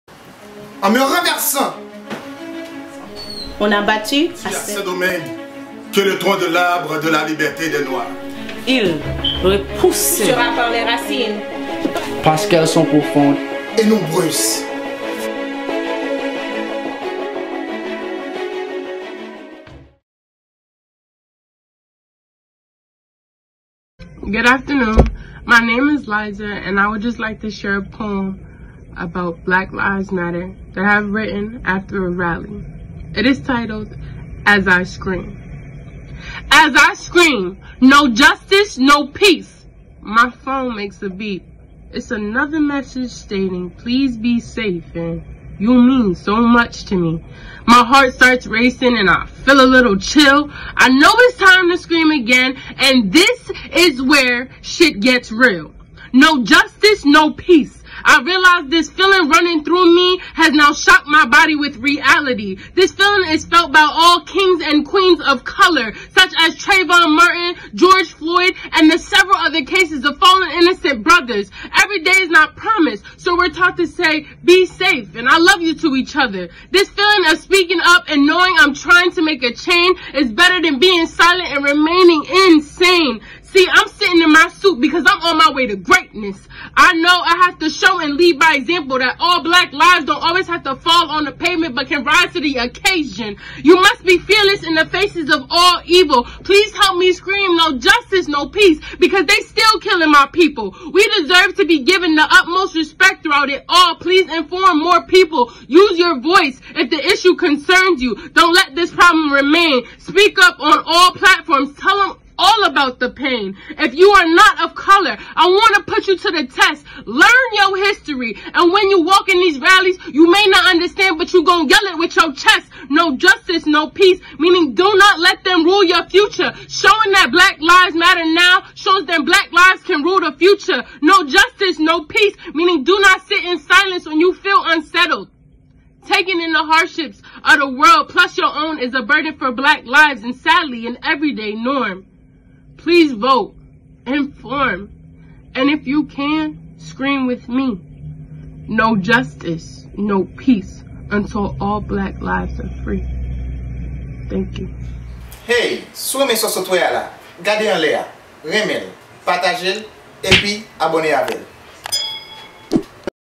As I Scream - Black Lives Matter Spoken Word
Roots1804 member shares poem inspired by today's tragic events.
"You can't separate peace from freedom because no one can be at peace unless he has his freedom."  -- Malcolm X
Say their names!

*Follow Liza on these platforms*